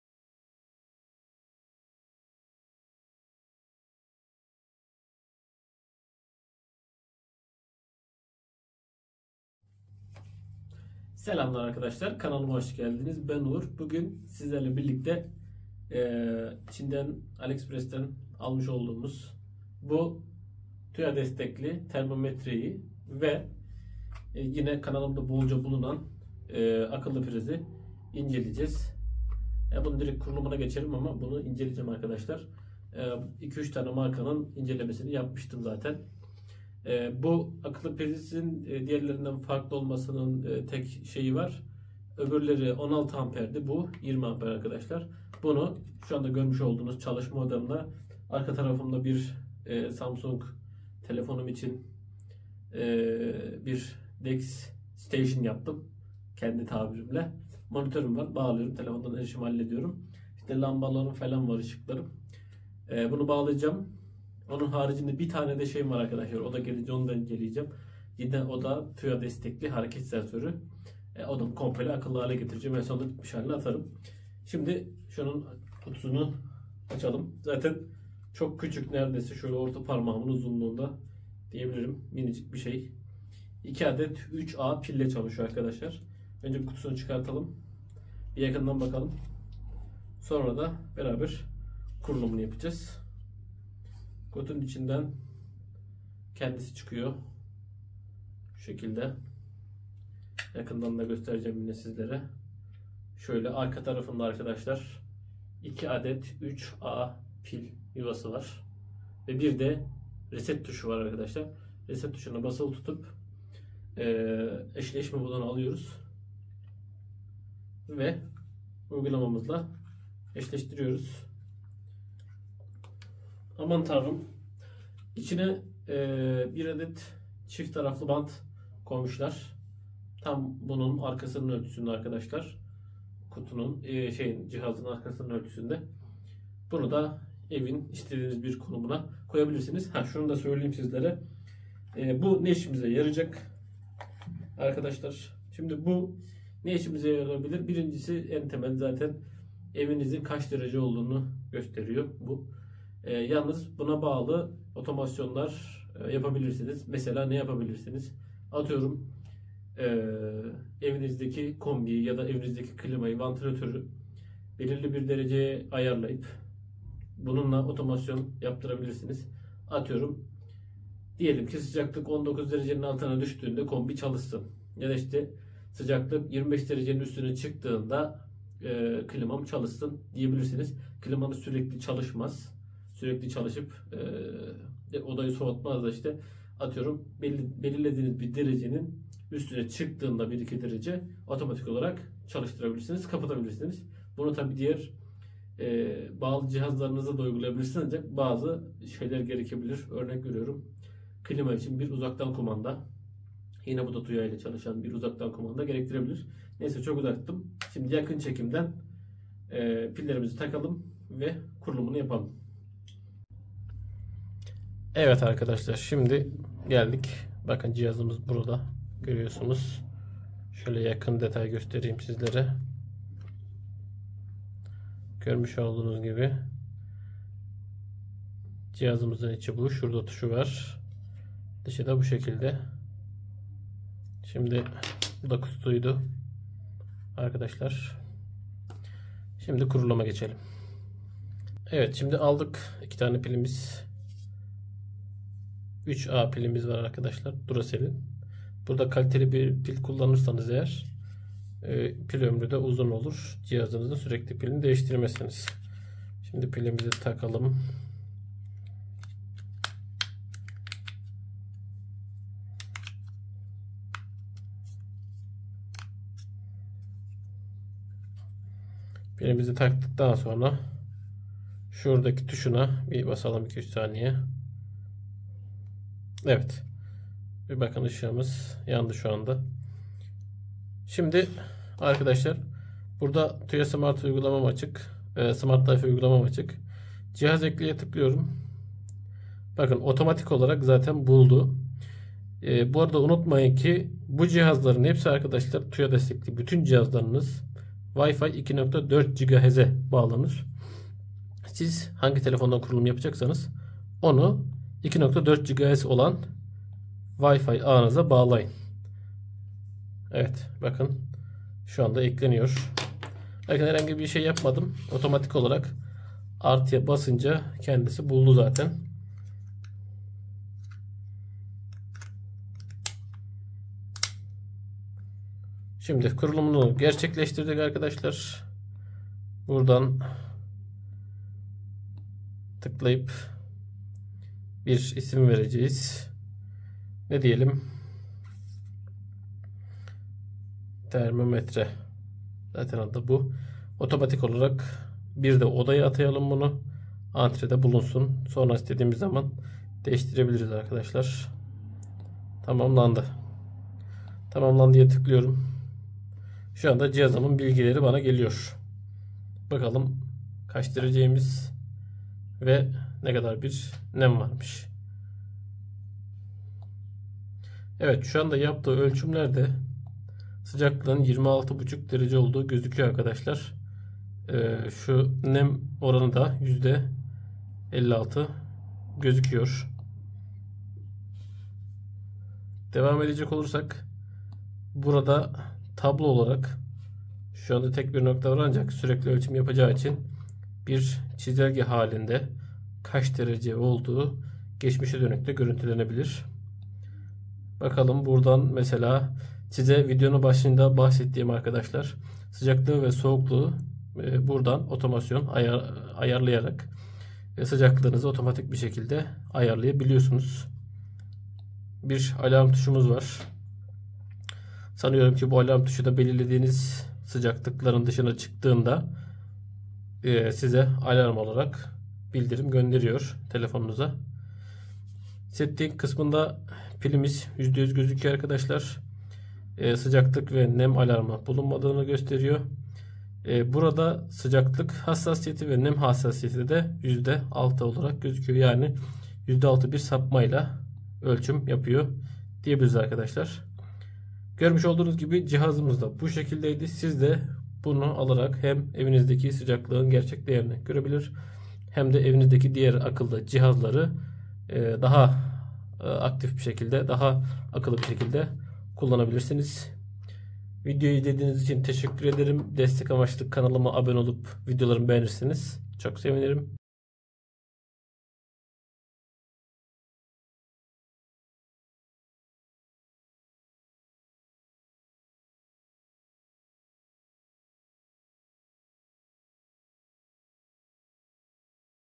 Tuya Destekli Akıllı Termometre İncelemesi | Evinizin Sıcaklığını Telefondan Görüntüleyin
Merhaba sevgili takipçilerim. Bu videoda sizlerle birlikte,  aliexpressten ilk indirimle almış olduğum vergiler dahil 100₺ ye gelen tuya destekli akıllı termometreyi inceliyoruz.